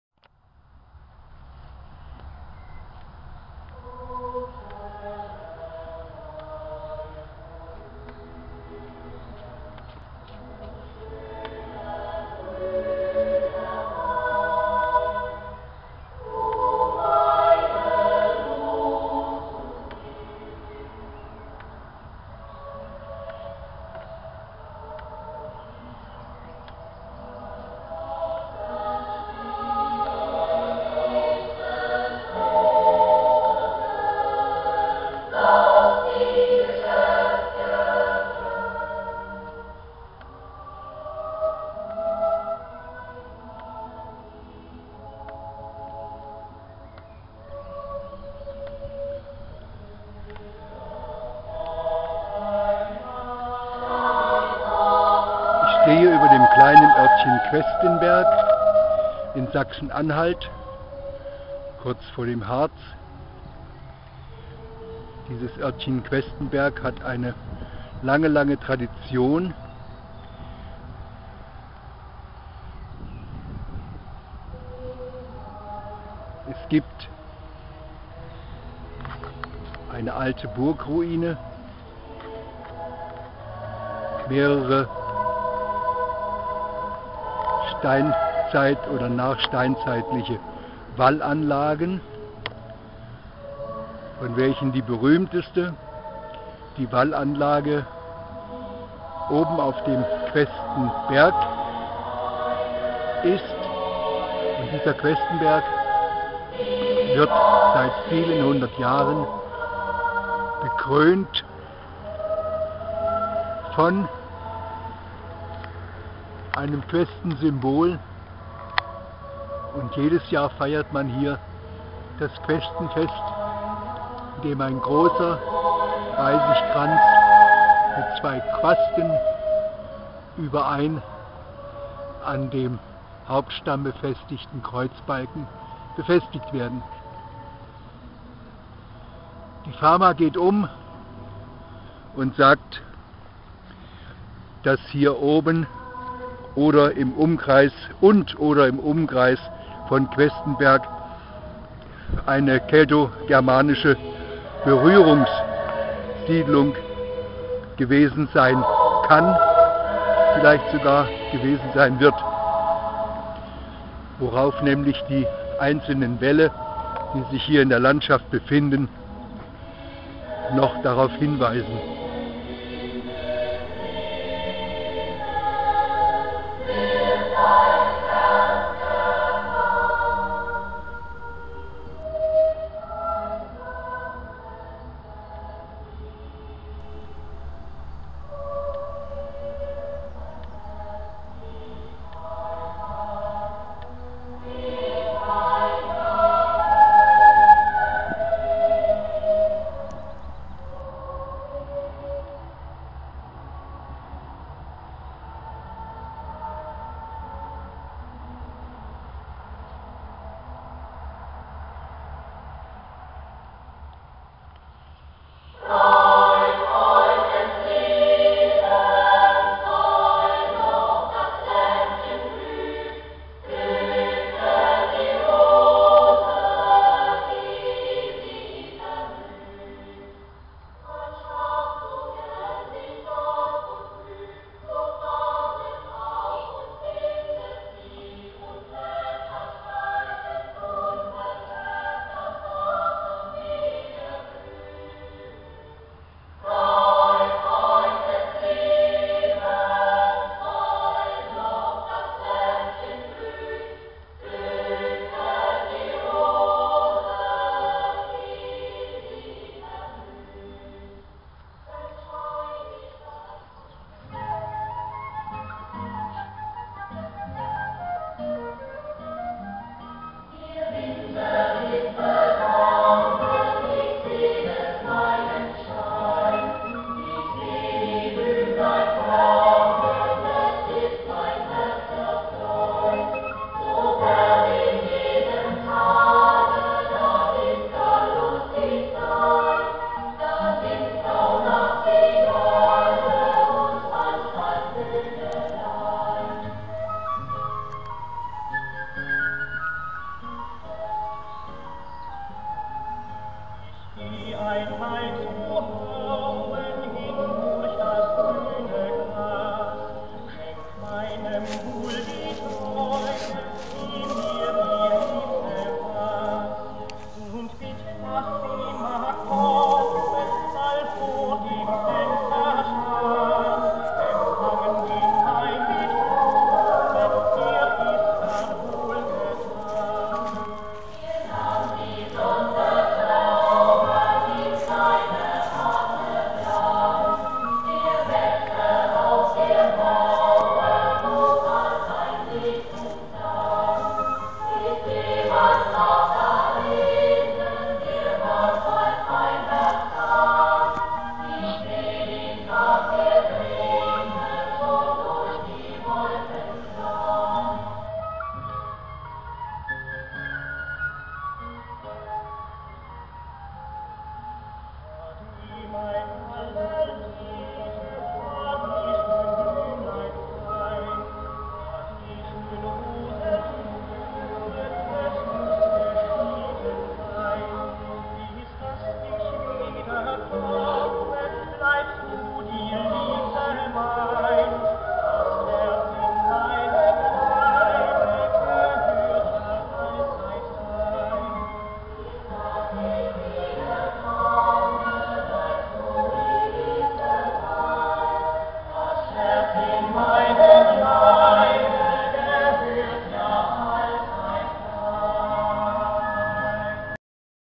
Von Questenberg das Beste - hoch überm Ort die Queste
Der kleine Ort Questenberg im Vorharz feiert seit Jahrhunderten das Questenfest, welches auf einer Begebenheit aus dem 13. Jahrhundert beruhen soll. Die jährliche Aufhängung eines riesigen Reisigkranzes, samt zweier Questen (Quasten) kann auch ein Jahreszeitenbrauch der dortigen Siedler in vorchristlicher Zeit gewesen sein - egal - ich kann nur meinen Zuschauern empfehlen, sich dieses Volksfest als Besucher anzuschauen. Einkehren kann man im Gasthaus "Zur Queste" im Dorfe selbst.
Im Nachbarort Agnesdorf begeht man seit vielen Jahrzehnten auch das Kirschblütenfest! Einzigartig schön! Viel Vergnügen allen